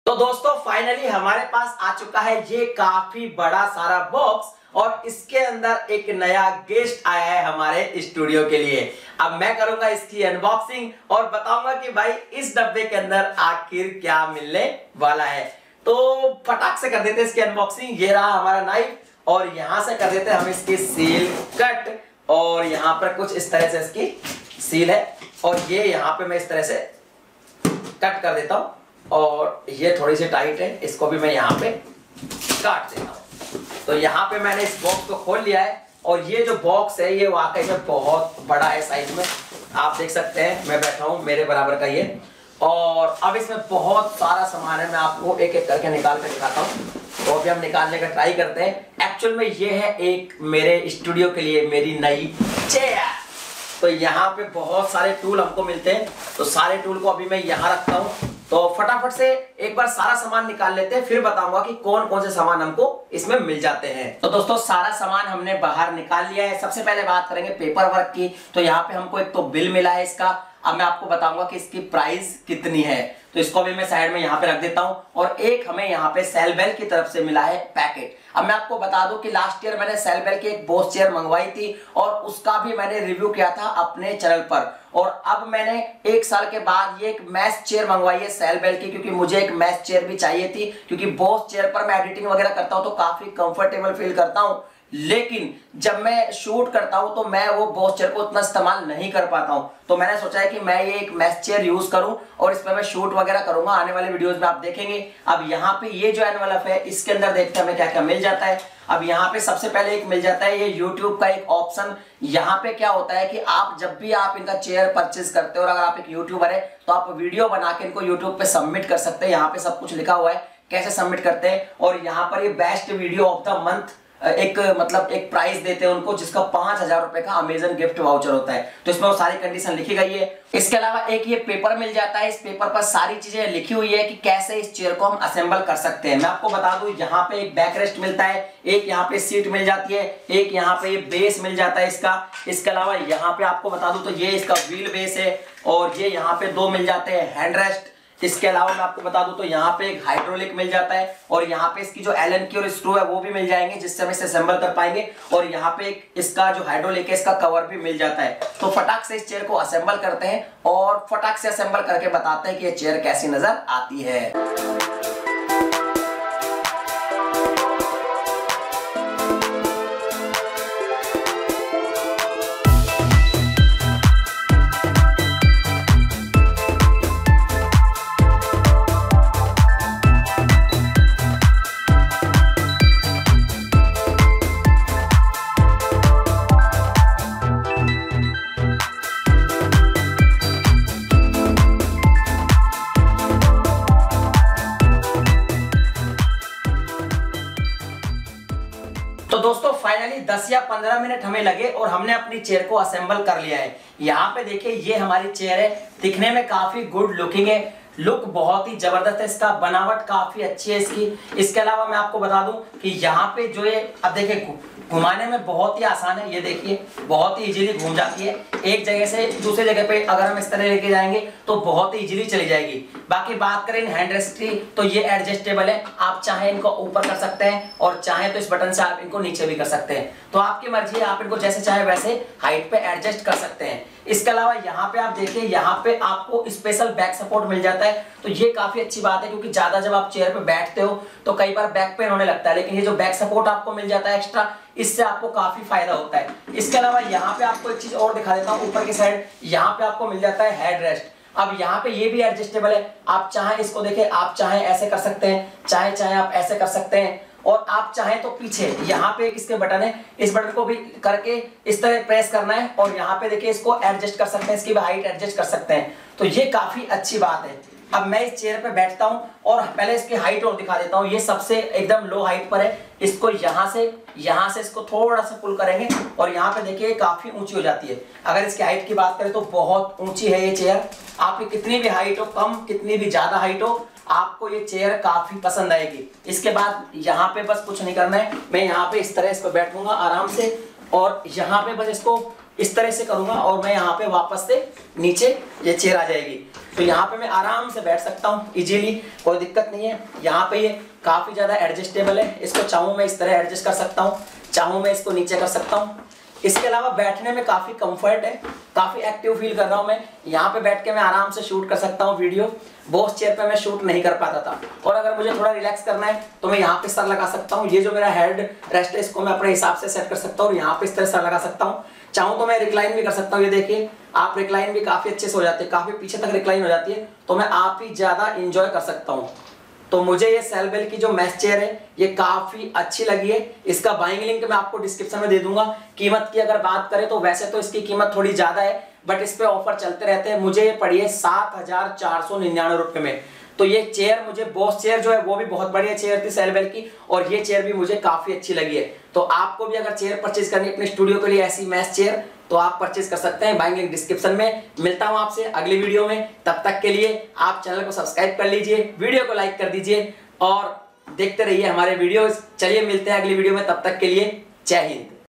My New Chair for My YouTube Studio | Best Office Chair in India | Cellbell C100
Dosto Aaj Ki Is Video Me Maine Ek Mesh Chair Ki Unboxing Ki Hai Jo Maine Apne YouTube Studio Ke Liye Liya Hai Ummeed Hai Aapko Ye Video Bahut Pasand Aayegi Thank you so much...

About this Chair-
1. Product Highlights: 2 inch thick padded seat for long hours comfort, Pneumatic Hydraulic for 5 inch seat Height adjustment, Breathable contoured Mesh back, Ergonomically designed parts such as: strong plastic Armrest and heavy duty metal Base.

2. Product Dimensions: It measures (18 inch) by (19 inch) by (47 to 51 inch) (LxWxH).

3. Maximum weight capacity: 105 Kgs.

4. Warranty: 1 year Limited Warranty as per this clear brand policy.